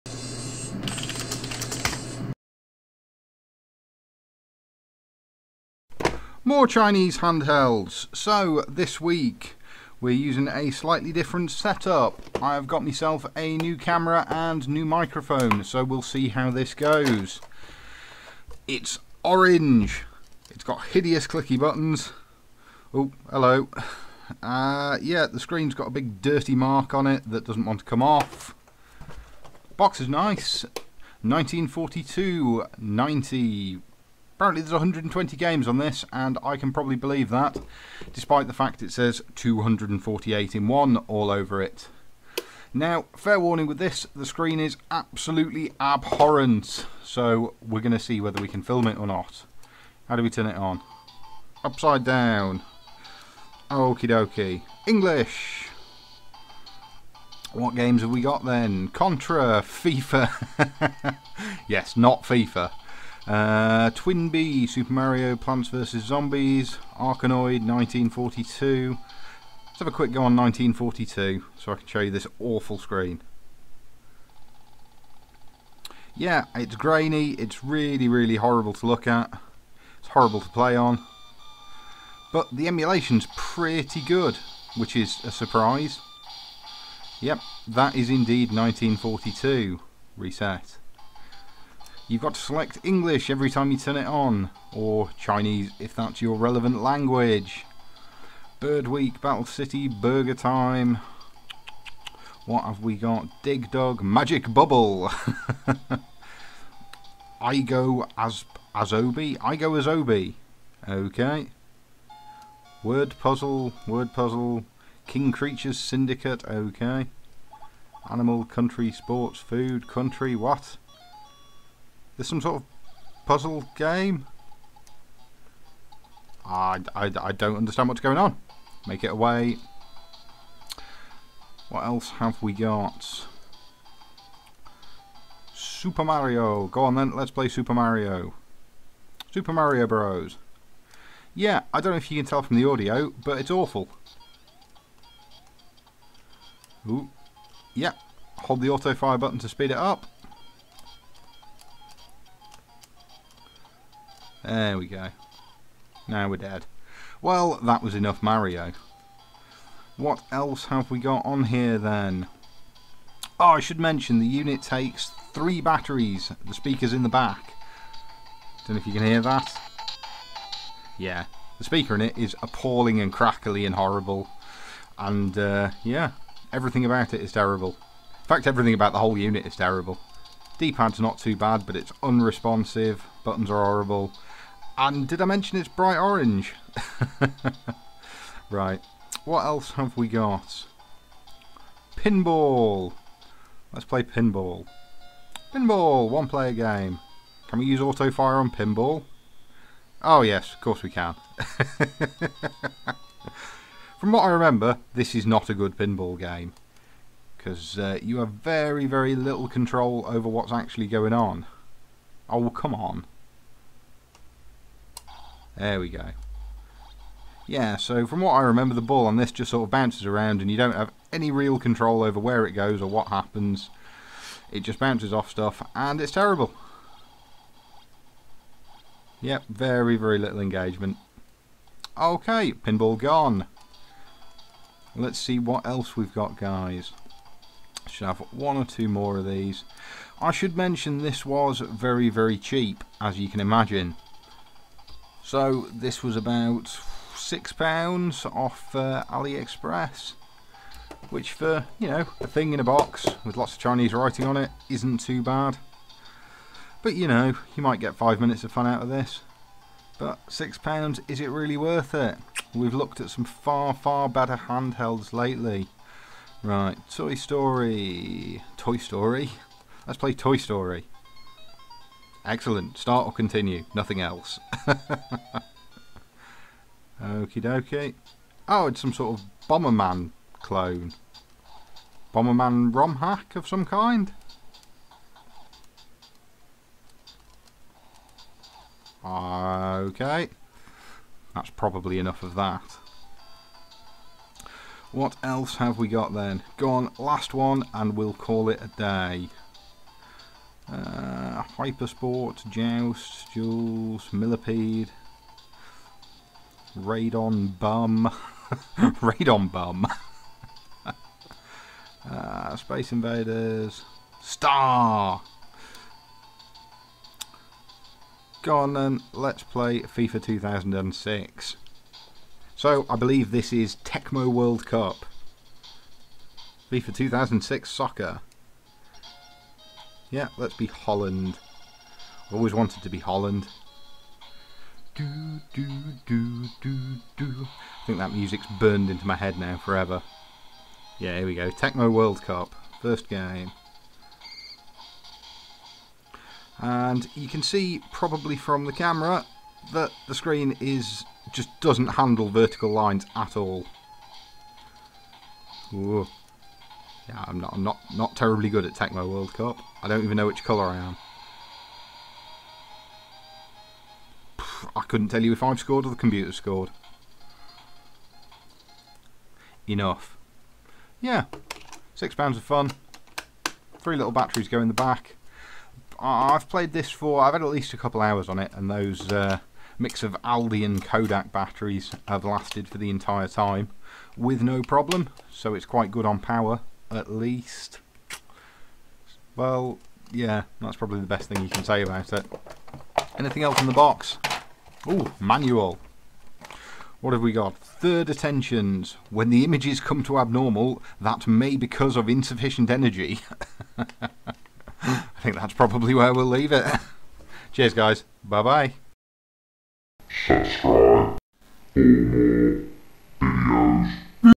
Cheapest Handheld NES Player | Coolbaby 298 in 1 Review
Cheapest NES handheld game player on the Market! Yes it's the Coolbaby 298 in 1 (although it only has 120 games, a lot of which are Rom hacks).
Y'no how much this thing was? £6. Seriously, £6 from Aliexpress. That was on sale though, looks like they're up to around £10 now.

Usual product blurb below:
Coolbaby RS-18 Portable Handheld Gaming Player Vedio Game Console Child Kids Boy Built-in 298 Games Classic Retro Games Consoles boy toy.
Specification:
Built-in 120 different classic games,The menu show 298 IN 1.
AAA battery and 1.8 inch display.
ABS materials, environmental protection, health
Child-friendly puzzle game, is also suitable for adult nostalgia
Color:Green,orange
 The product is not suitable for children under 36 months old.
When the game player appears raucous sound.slow speed.flickering screen,carshed,please replace the battery.
If the screen doesn't display,please make sure the game's power switch is turned on.

That's word for word the rubbish from the product page.